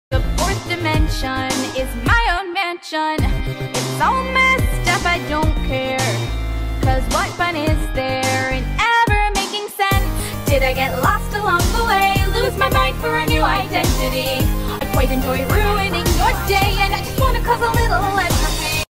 The forth demission is my own mansion 💅
Did I get lost along the way? Lose my mind for a new identity 🥱

Game: Gacha Club and Gacha life
Edited with: CapCut and VITA

Characters: My old Gacha character, my new Gavha character, Poppy and Mommy long legs

Old trend I think-

I had fun making this!!

Comment anything you wanna see my do on my channel!

Cya next time!